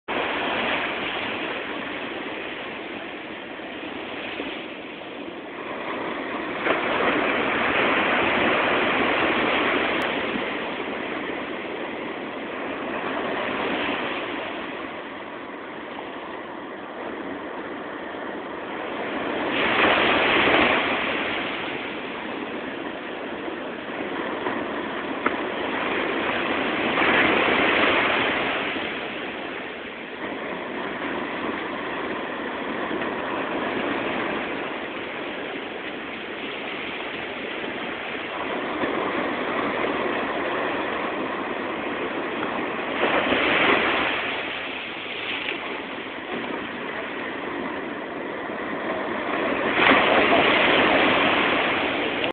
Watching the birds feed in the tide 2
Bird watching from a pier in Southern California.  This was recorded with a Samsung Flight 2 cell phone.